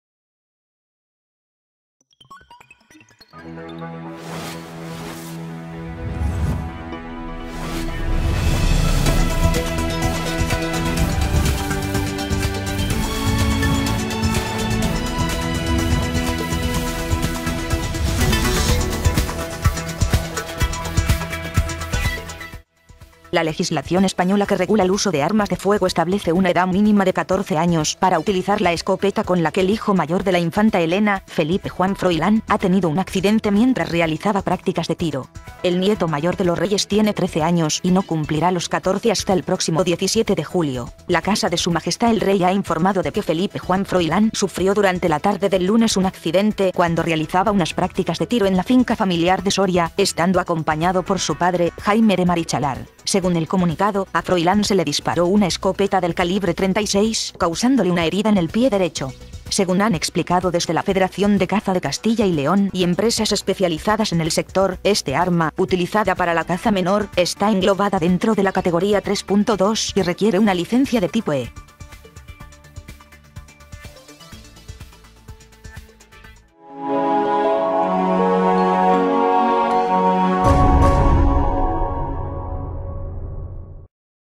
FROILÁN MANEJABA UN ARMA NO PERMITIDA A MENORES DE 14 AÑOS
La legislación española que regula el uso de armas de fuego establece una edad mínima de 14 años para utilizar la escopeta con la que el hijo mayor de la infanta Elena, Felipe Juan Froilán, ha tenido un accidente mientras realizaba prácticas de tiro. El nieto mayor de los Reyes tiene 13 años y no cumplirá los 14 hasta el próximo 17 de julio.

La Casa de Su Majestad el Rey ha informado de que Felipe Juan Froilán Marichalar Borbón sufrió durante la tarde del lunes un accidente cuando realizaba unas prácticas de tiro en la finca familiar de Soria estando acompañado por su padre, Jaime de Marichalar.

Según el comunicado, a Felipe Juan Froilán se le disparó una escopeta de pequeño calibre (calibre 36) causándole una herida en el pie derecho. Según han explicado desde la Federación de Caza de Castilla y León y empresas especializadas en el sector, este arma, utilizada para la caza menor, está englobada dentro de la categoría 3.2 y requiere una licencia de tipo E.

Ley vigente
El Ministerio del Interior contempla autorizaciones especiales para aquellos menores de edad que deseen utilizar este tipo de escopetas, aunque siempre a partir de los 14 años y que vayan acompañados de personas mayores de edad con licencia de armas que previamente se hayan comprometido a acompañarlos y vigilarlos en cada cacería.

En concreto, el artículo 109 del real Decreto 137/1993 del 29 de enero, por el que se aprueba el Reglamento de Armas, establece que el uso de armas de categoría 3.2. para la caza está restringido a los mayores de 14 años, siempre que se encuentren en posesión legal de una autorización especial de uso de armas para menores y vayan acompañados de personas mayores de edad con licencia de armas.

El nieto mayor de los Reyes, que no cumplirá los 14 años hasta el próximo 17 de julio, ha tenido que ser intervenido en la clínica Quirón de Madrid esta madrugada para ser sometido con éxito a una limpieza quirúrgica.

El Ministerio del Interior establece asimismo las sanciones por el uso irregular de armas de fuego y contempla multas económicas a partir de los 300 euros para su uso sin licencia, así como la incautación de las armas.